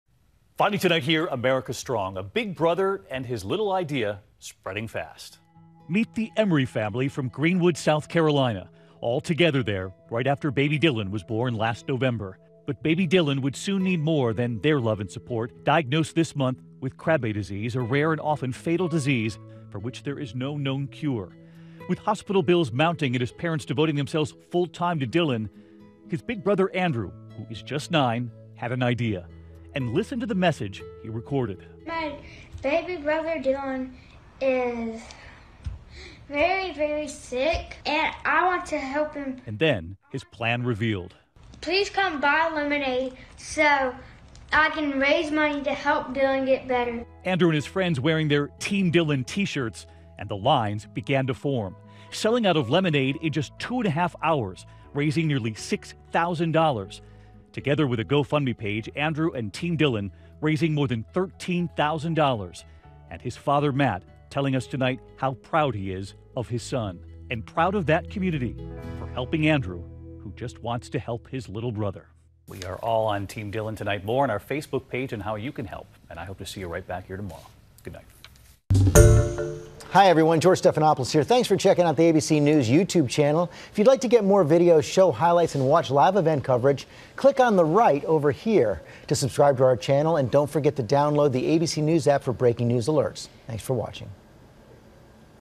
Boy raises nearly $6k selling lemonade to help with brother's medical bills
He also sold 888 Team Dylan shirts for his sibling who was diagnosed with a rare disease.